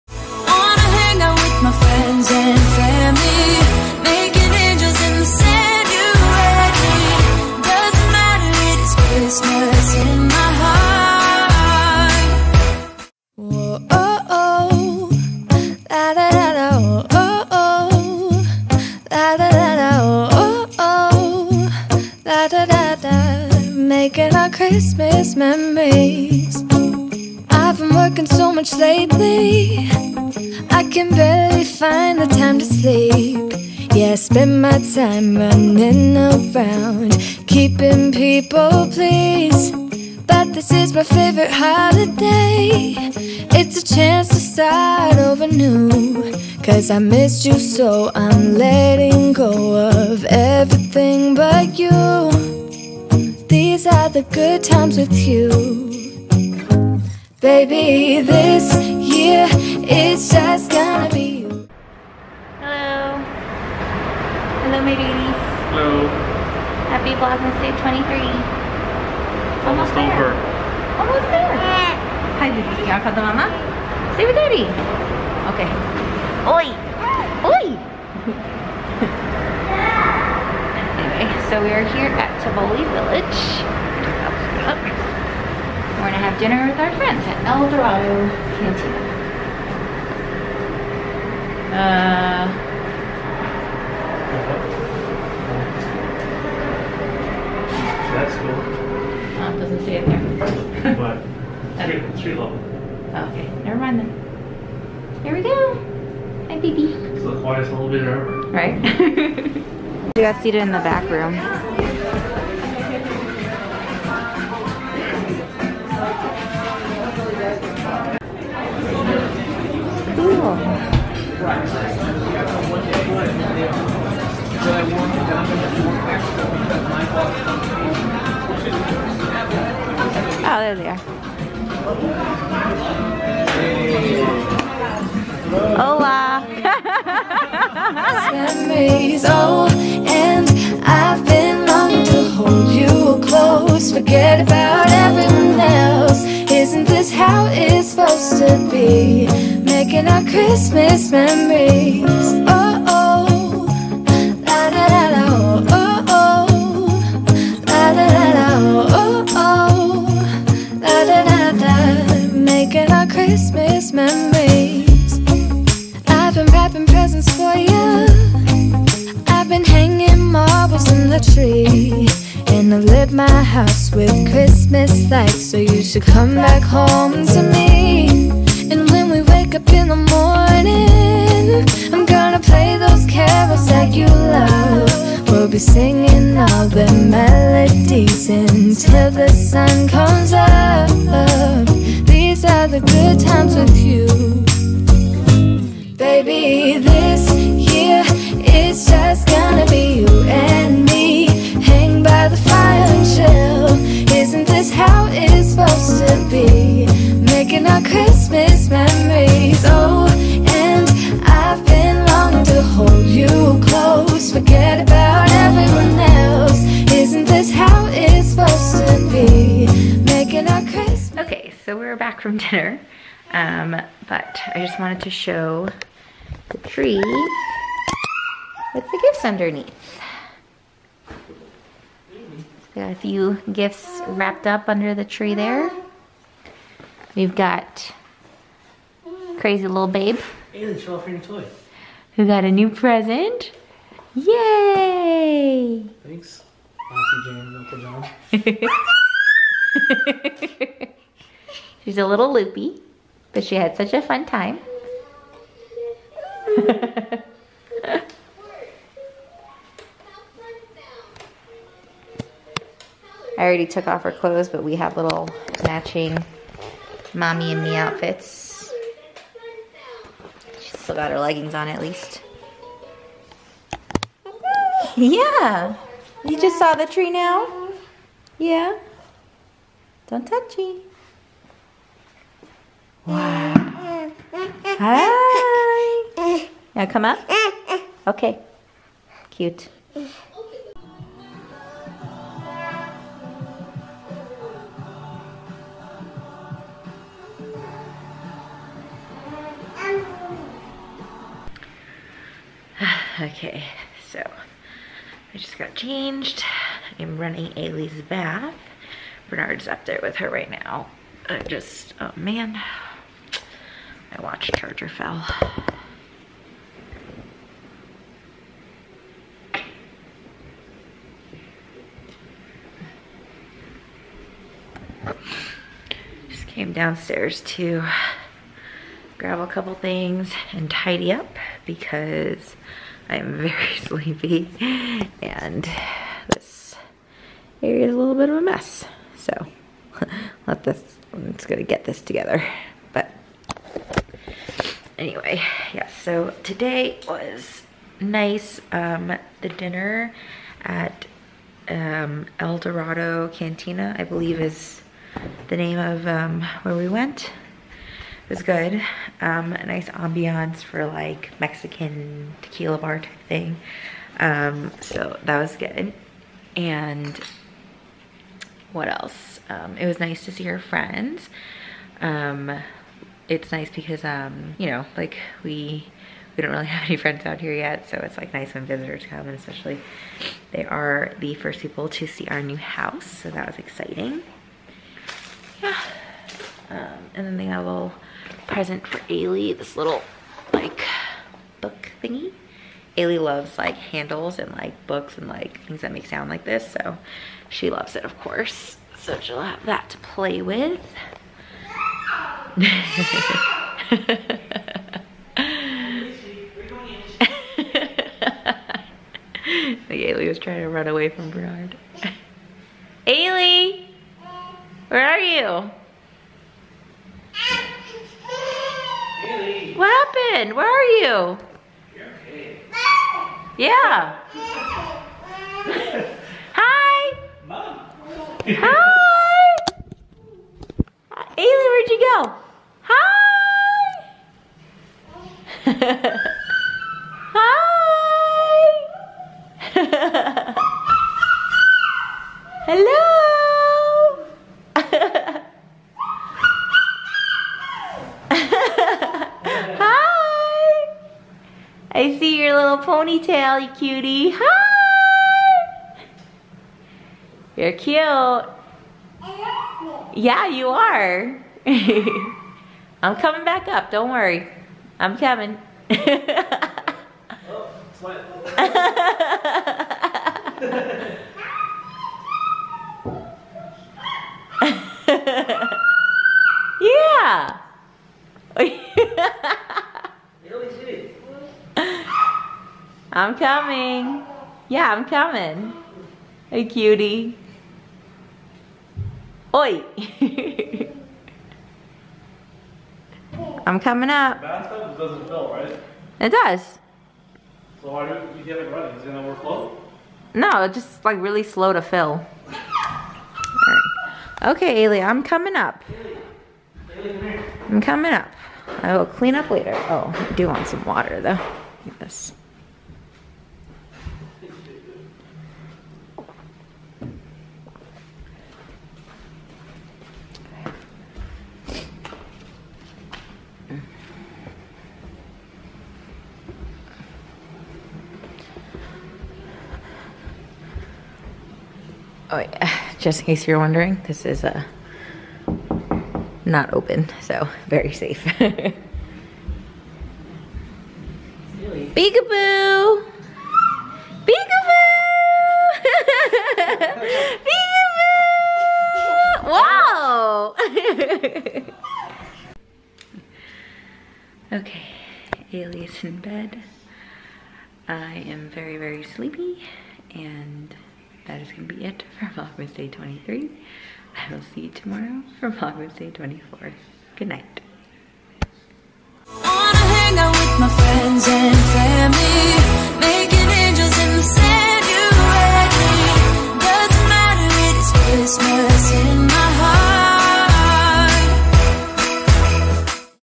VLOGMAS 2021 DAY 23: Wrapping Presents, Tableside Guacamole + Loopy/Cute Toddler
On the 23rd day of Vlogmas, I finally wrapped some presents and had dinner with friends in town at El Dorado Cantina in Tivoli Village.

Advent Calendar